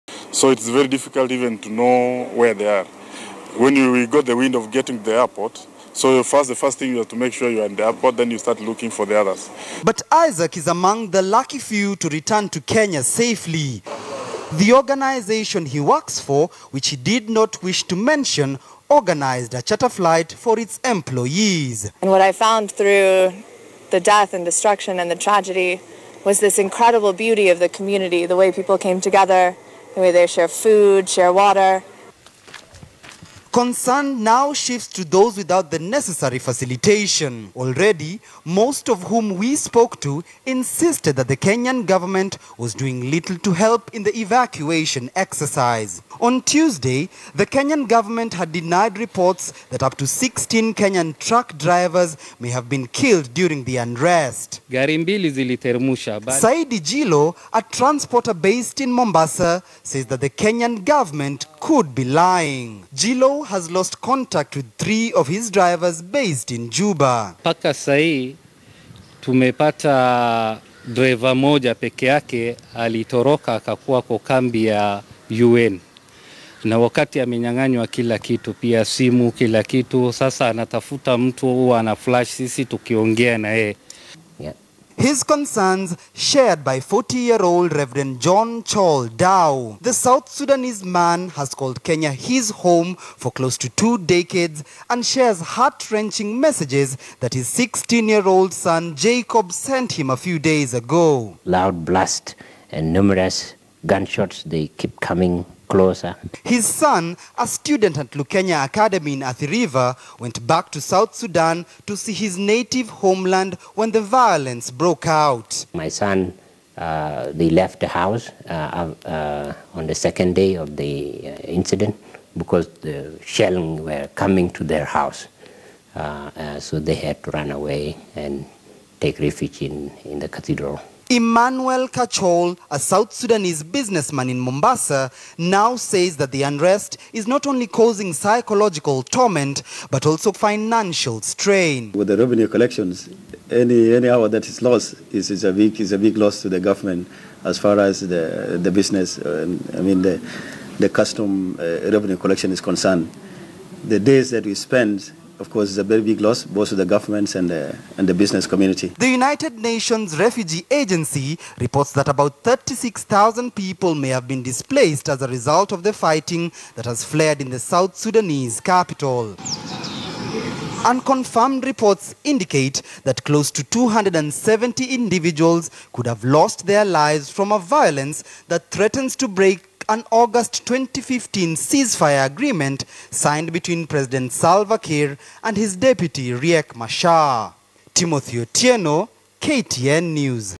South Sudanese capital’s airport reopened in the wake of the ceasefire
An uneasy calm has returned to the South Sudanese capital of Juba following days of unrest. The relative unease saw the capital’s airport reopened even as Kenyans struggled to return home in the wake of the ceasefire. Timothy Otieno reports.